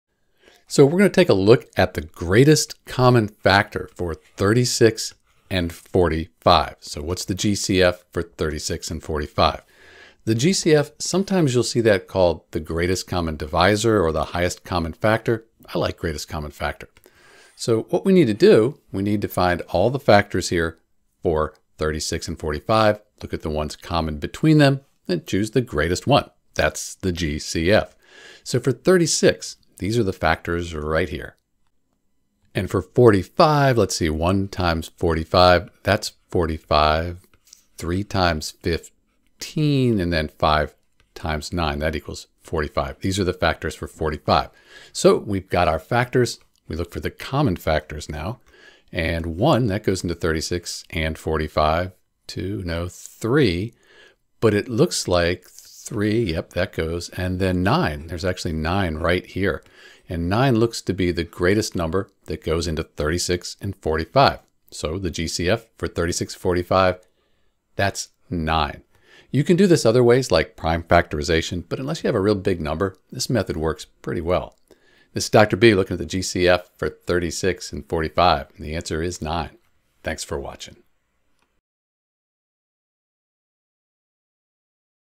How to Find the Greatest Common Factor for 36 and 45
• The factors of 36 are the numbers that can evenly divide 36. In this case, they are 1, 2, 3, 4, 6, 9, 12, 18, and 36.

• The factors of 45 are the numbers that can evenly divide 45. In this case, they are 1, 3, 5, 9, 15, and 45.

Common Factors: 1, 3, 9

Note: The GCF represents the largest number that can evenly divide both 36 and 45.